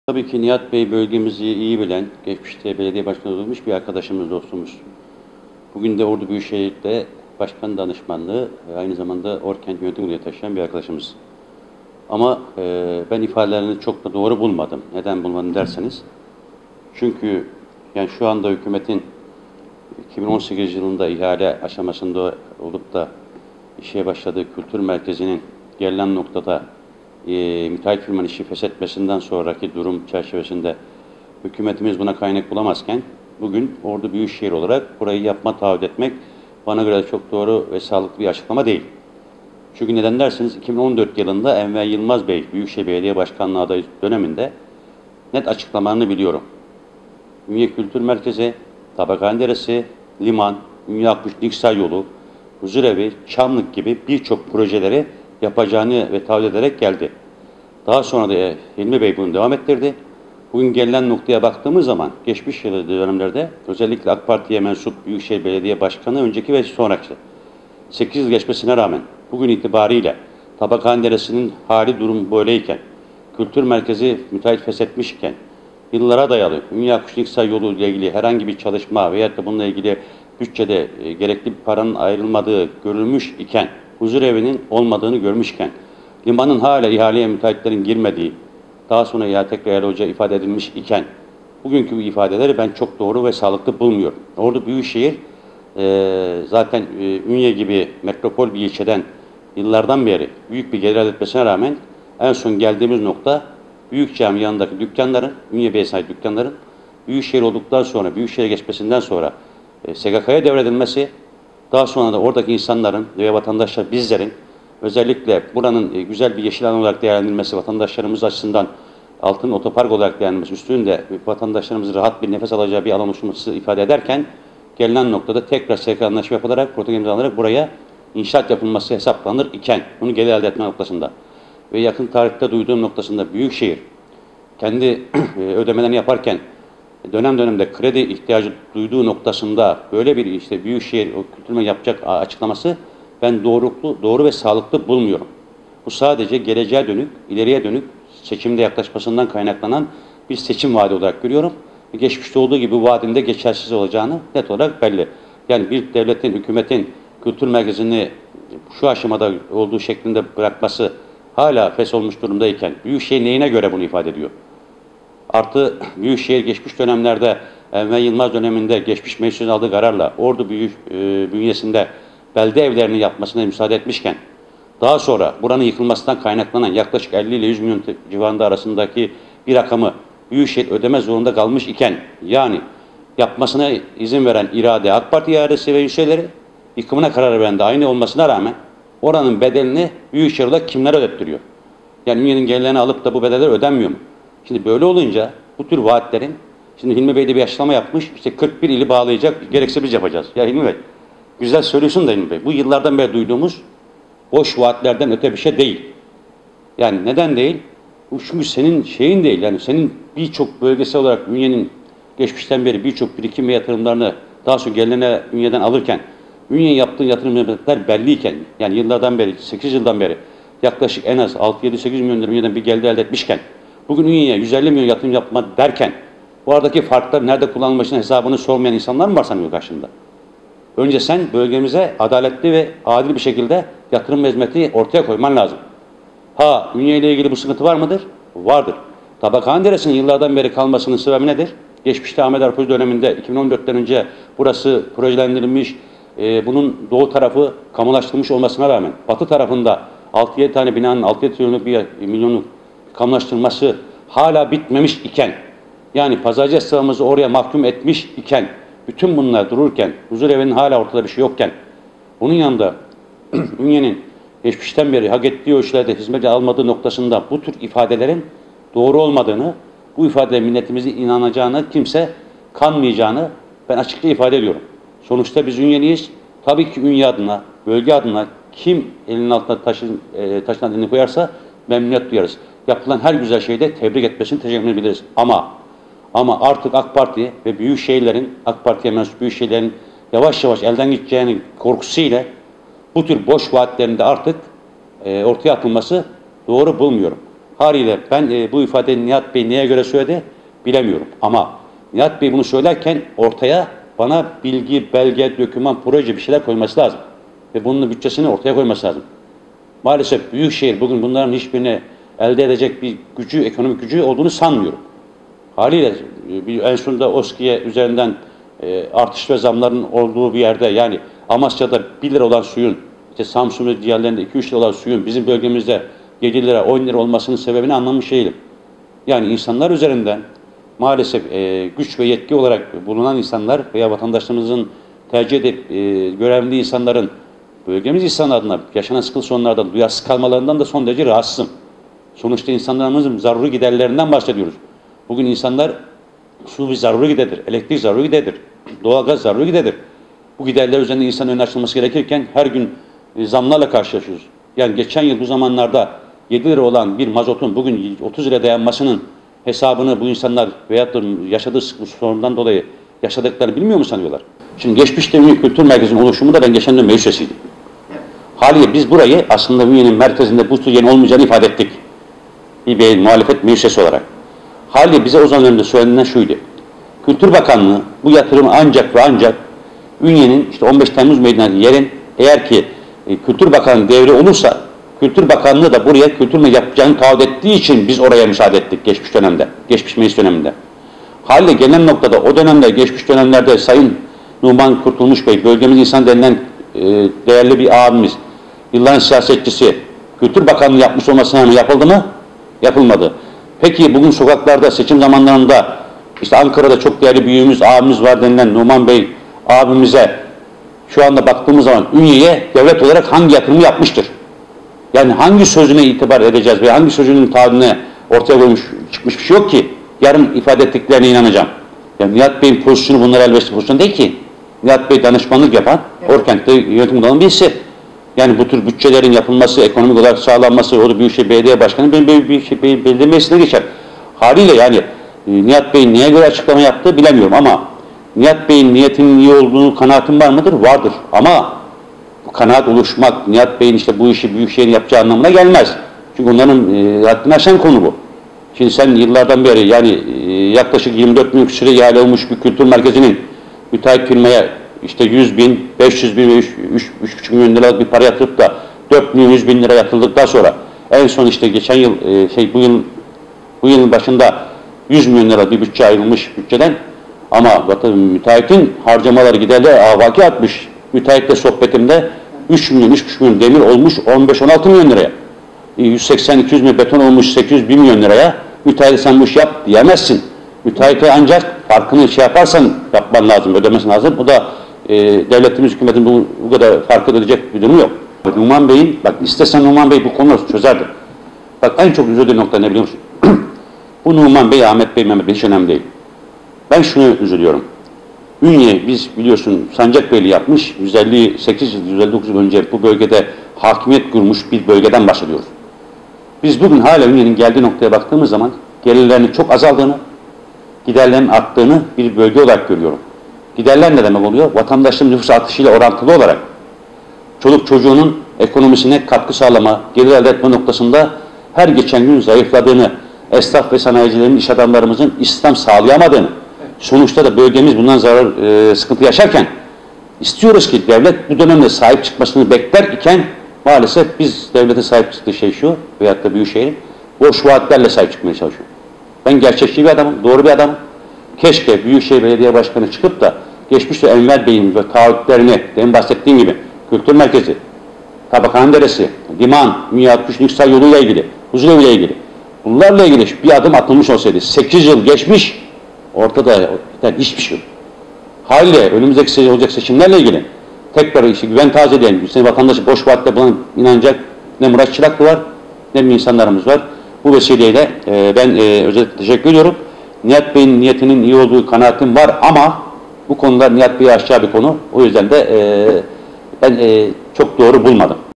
İyi Partili Çıraklı: "Büyükşehir 8 Senedir Ünye'ye Sahip Çıkmadı!"
Adres: Liseler Mahallesi Santral Sokak No:3 Ünye/Ordu / ORDU